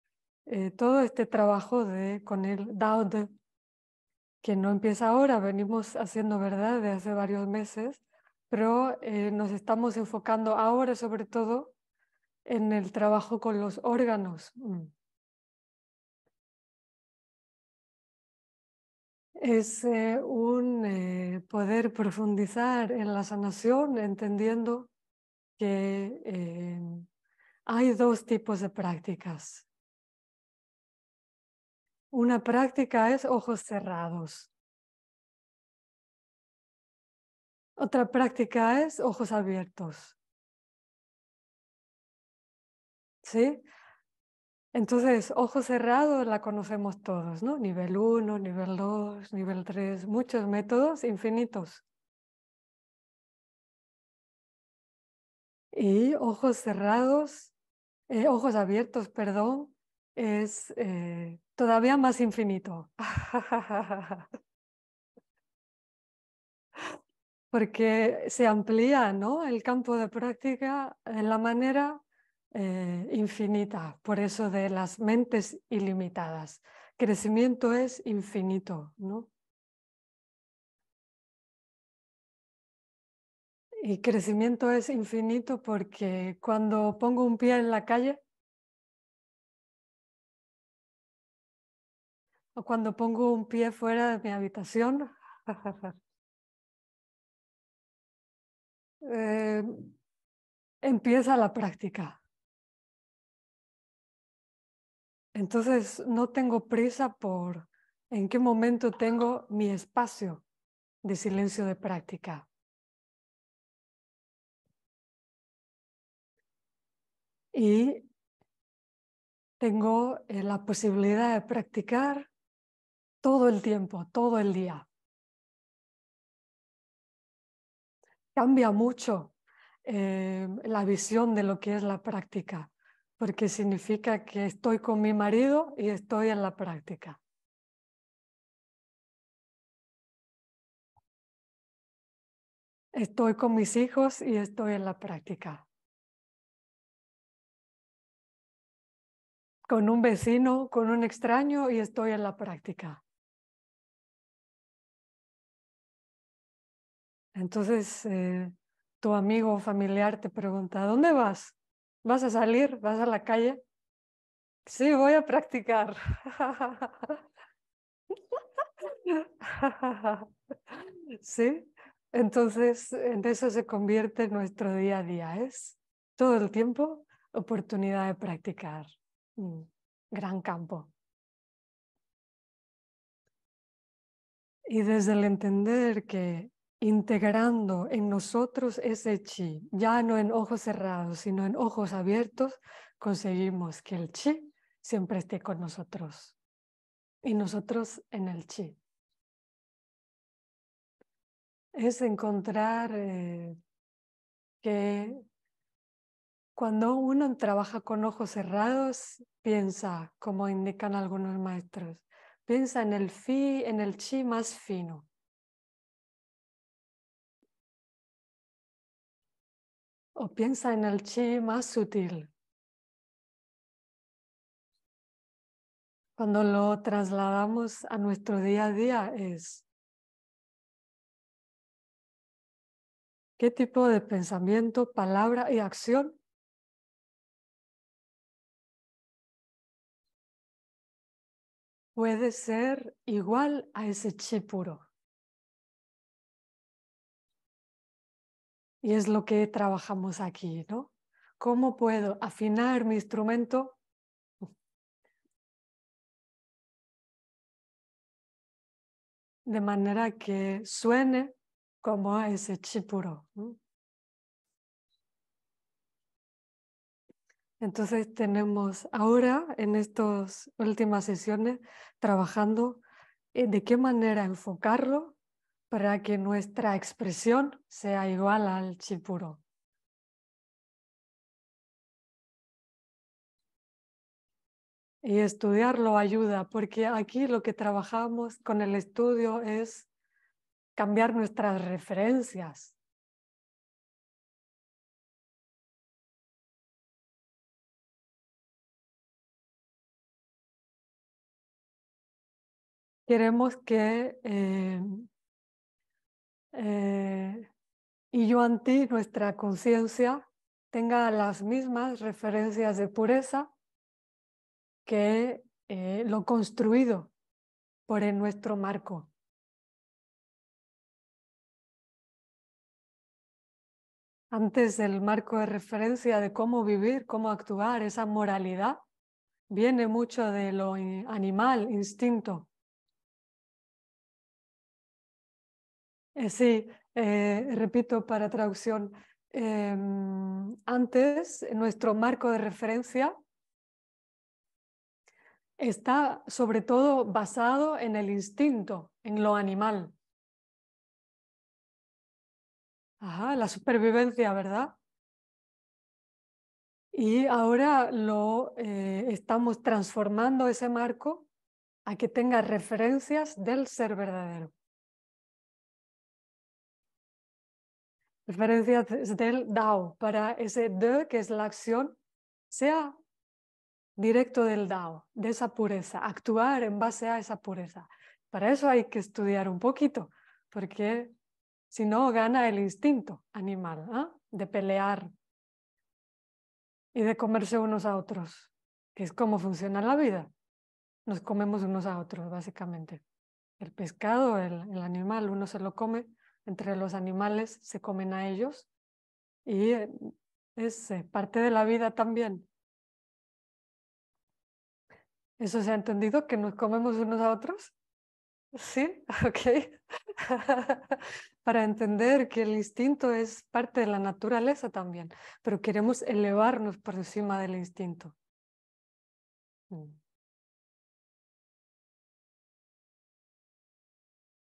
La expresión del qi más puro
Práctica del Dao De: cómo llevar la práctica a otro nivel integrándola en uno mismo y en la vida cotidiana.

Escuela One Light
Abarcamos los diferentes aspectos del ser humano, cuerpo, emociones, mente, Ser, para su sanación, reequilibrio y desarrollo, para su armonización interior y plenitud en el mundo.